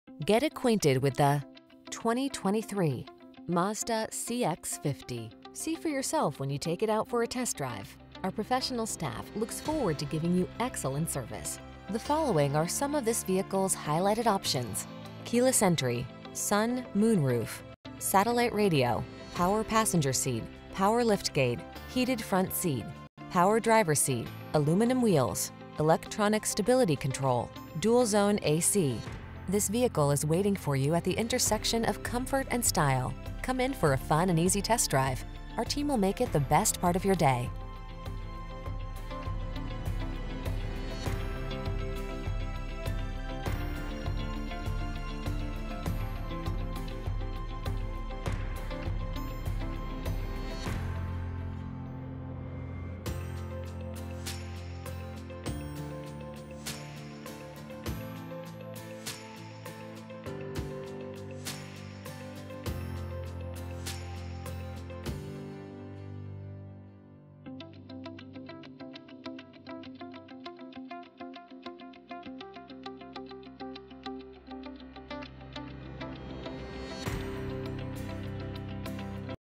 2023 Mazda CX-50 Arvin, Oildale, Shafter, Wasco, Bakersfield, CA 70507
Jet Black Mica New 2023 Mazda CX-50 available in Bakersfield, CA at  Bakersfield Mazda. Servicing the Arvin, Oildale, Shafter, Wasco, CA area.
2023 Mazda CX-50 2.5 S Premium Plus Package - Stock#: 70507 - VIN#: 7MMVABEM0PN115639

Bakersfield Mazda
2900 Cattle Dr